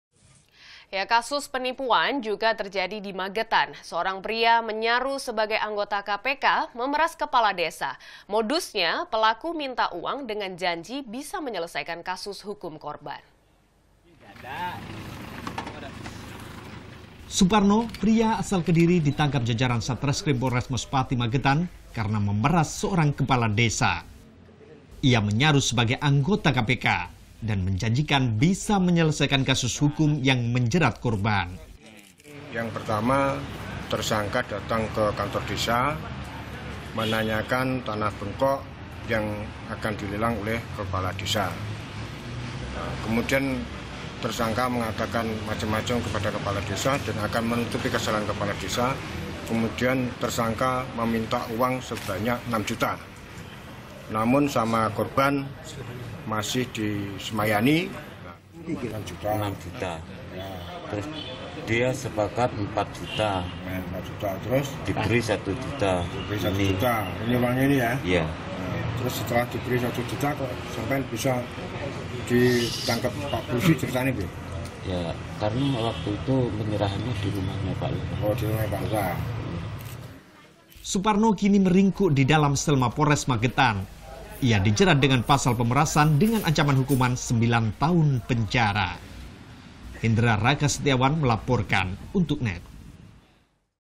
NET. JATIM - KPK GADUGAN DIBEKUK APARAT
** NET. JATIM **
Kasus Penipuan Juga Terjadi Di Magetan. Seorang Pria Menyaru Sebagai Anggota Kpk Memeras Kepala Desa. Modusnya, Pelaku Minta Uang Dengan Janji Bisa Menyelesaikan Kasus Hukum Korban.